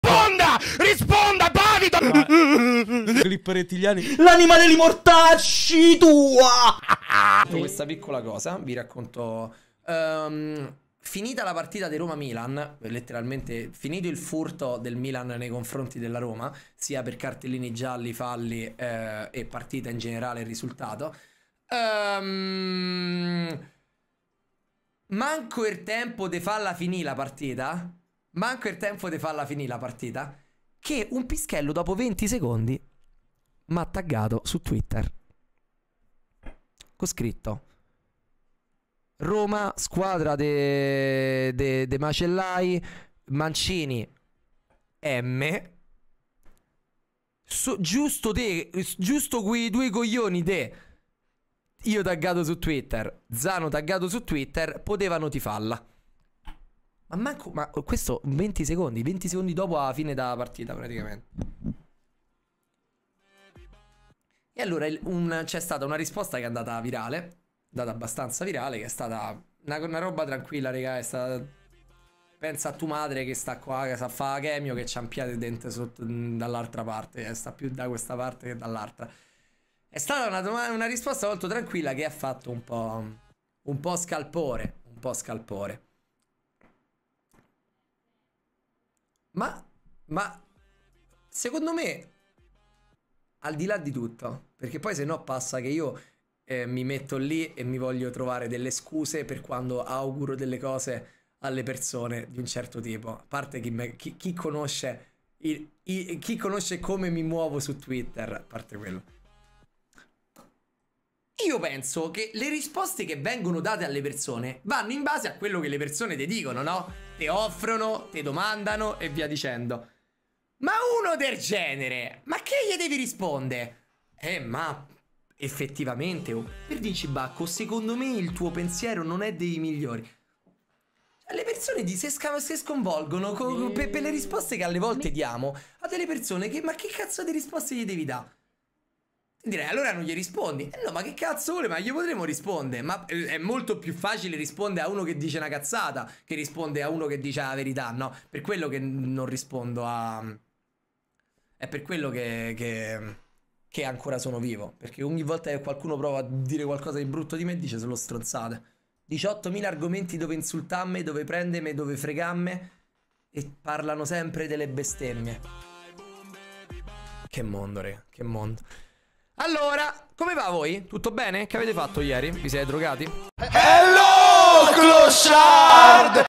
BLUR RISPONDE ALLE CRITICHE PER IL TWEET IN RISPOSTA AD UN UTENTE CONTRO LUI E ZANO DOPO ROMA MILAN
BLUR RISPONDE ALLE CRITICHE PER IL TWEET IN RISPOSTA A UN UTENTE CONTRO LUI E ZANO DOPO ROMA MILAN .